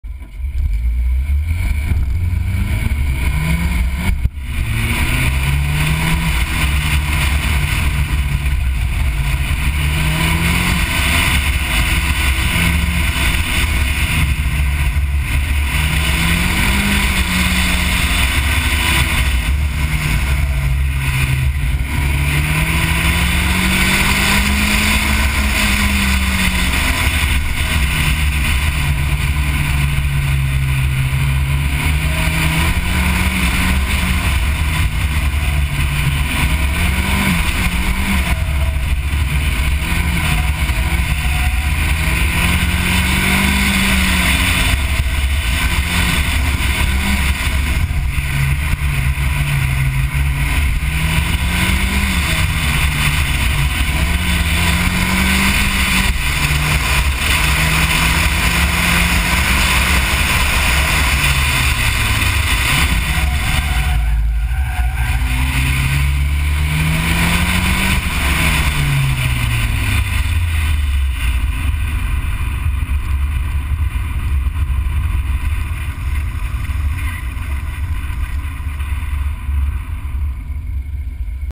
MS3 at CMP AutoX
Mazdaspeed 3 AutoXing at Carolina Motorsports Park.